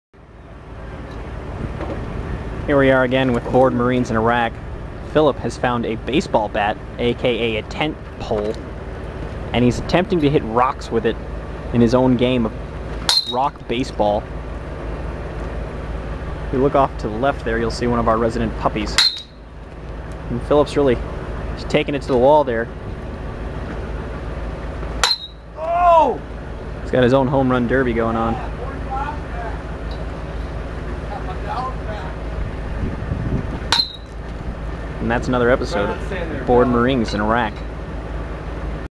Bored Marines in Iraq: Episode 6, Home Run Derby
One of the unavoidable aspects of being in the military is occasional, long fits of boredom. While this does happen frequently back at home, it is far more pronounced while deployed. Thus exists the l